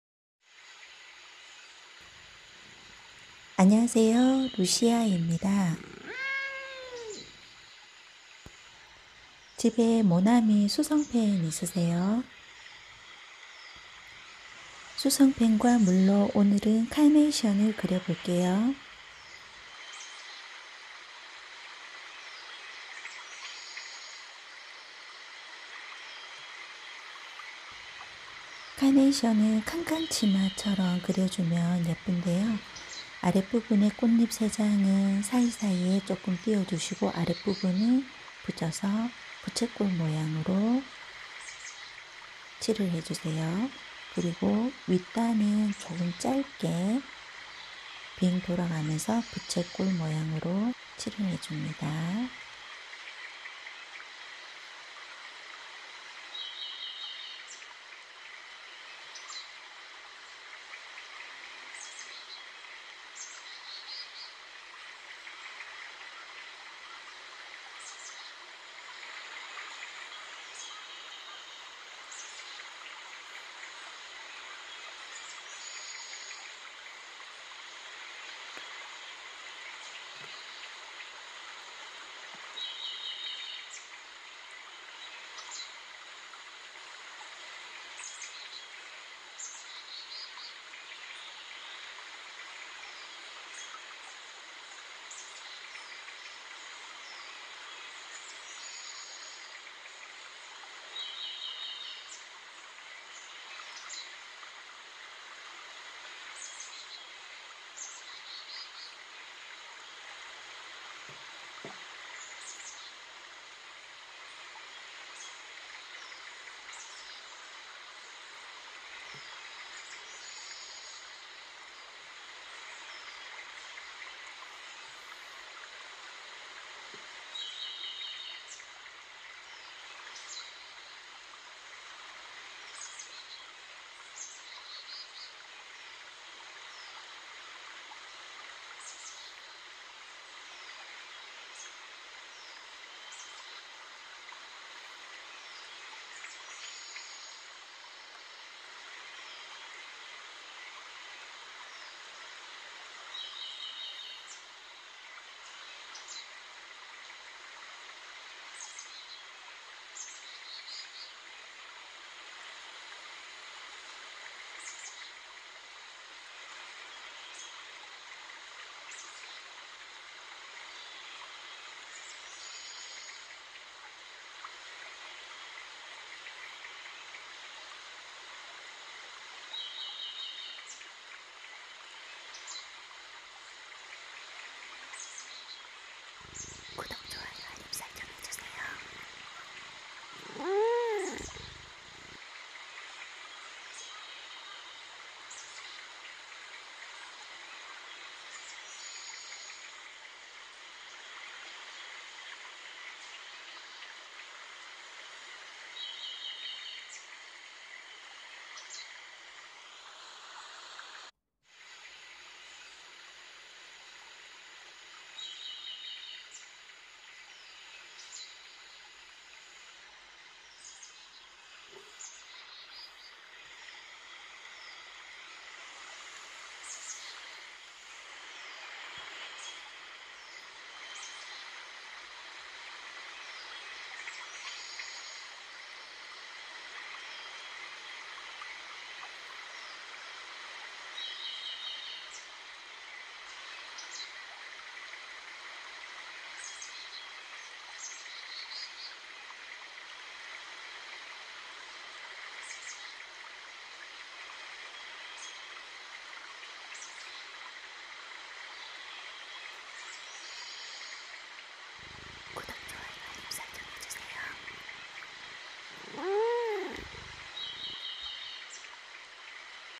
모나미수성펜 있으세요?어버이날
안녕하세요 루시아입니다

모나미수성펜 있으세요? 카네이션을 그려서 어버이날 스승의날 손편지는 어떠세요?
준비물은 내돈내산 모나미수성펜, 물을 준비해주시고 시중에서 판매하는 물이 잘 번지는 수채화용 엽서를 준비해주세요